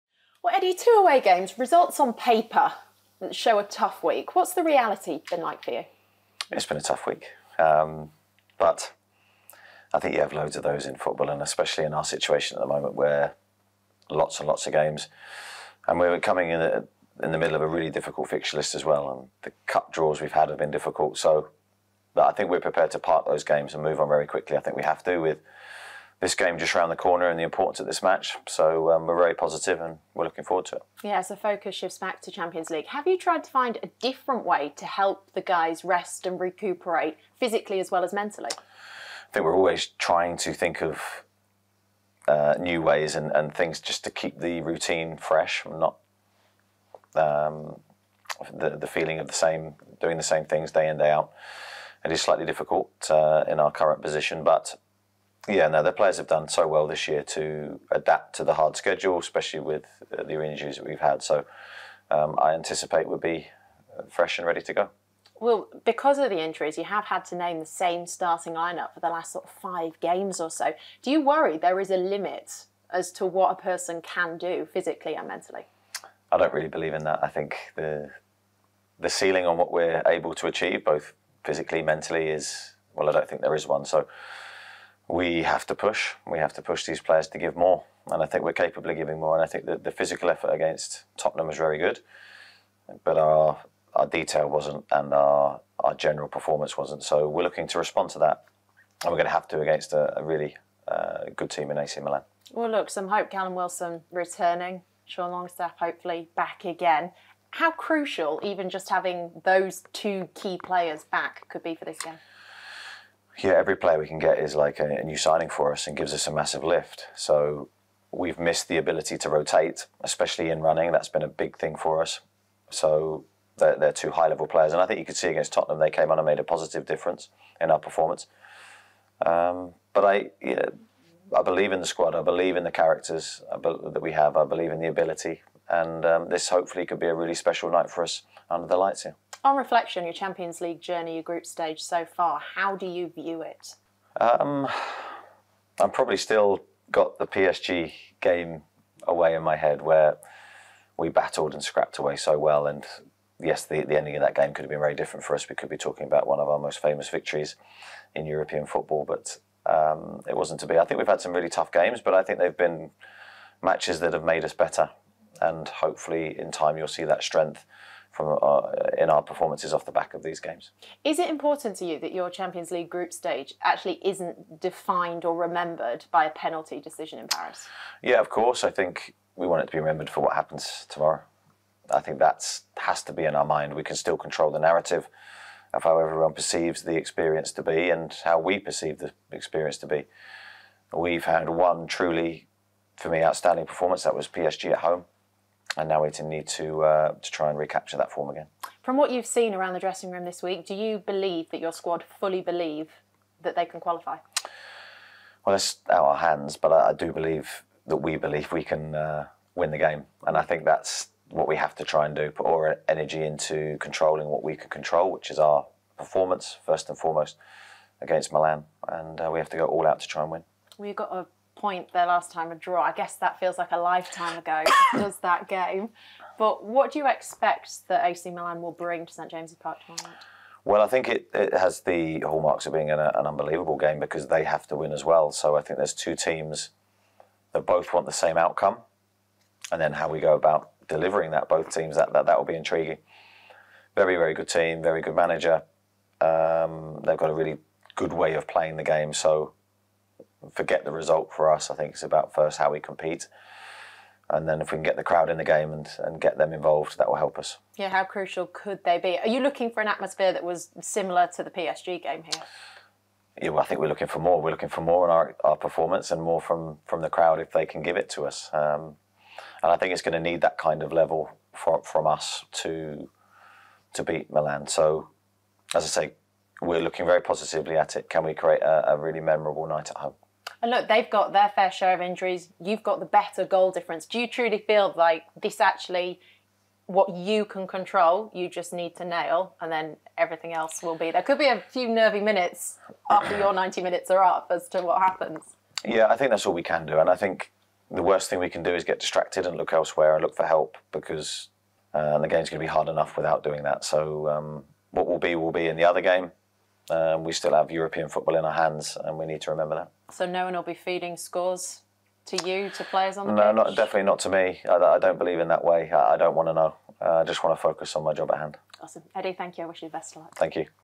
"I BELIEVE IN THE SQUAD" 🙏 | Eddie Howe reflects after a challenging week | Newcastle vs AC Milan
After what Eddie Howe described as a "tough week", he insists that Newcastle can deliver the performance required to secure a final 16 spot in the Champions League.